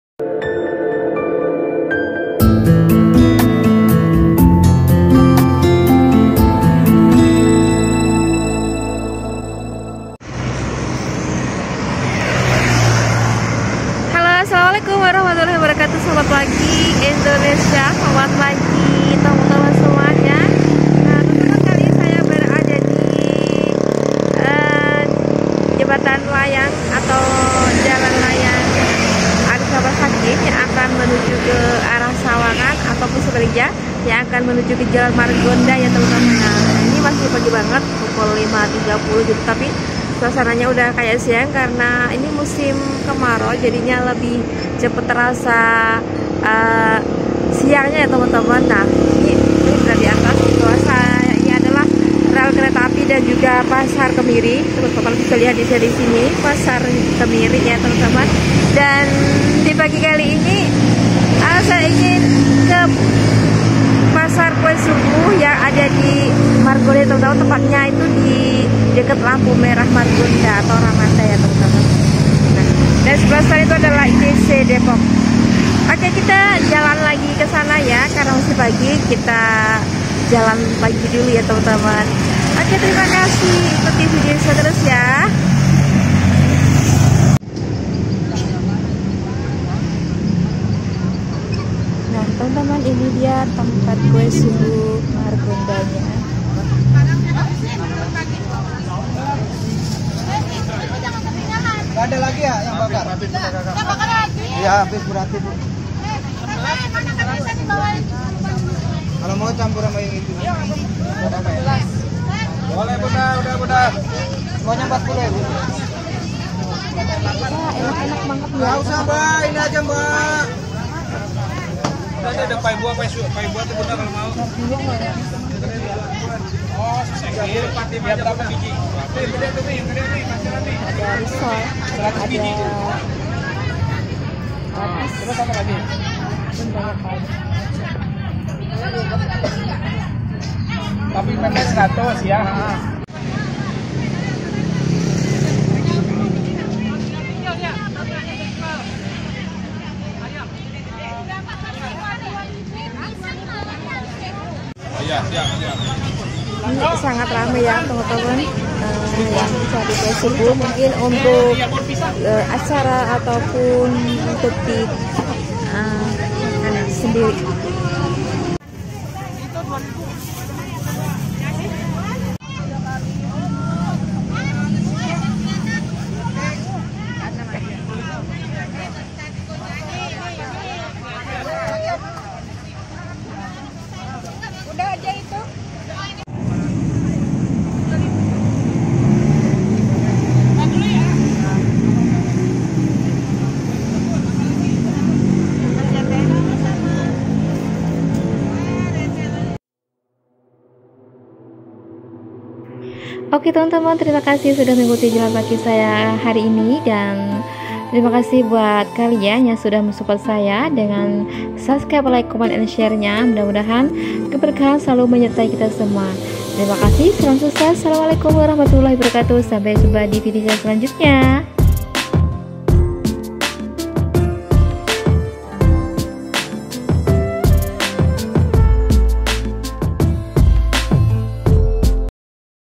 Enaaak semuaanya,Jajanan subuh margonda depok#kuetradisional
Assalamualaikum,dividio kali ink saya jalan2 pagi ketempat jajanan subuh margonda depok.
semoag vidio ini bermanfaat bagi warga depok dan sekitarnya yang mau beli jajanan subuh g usah jauhw kepasar senen.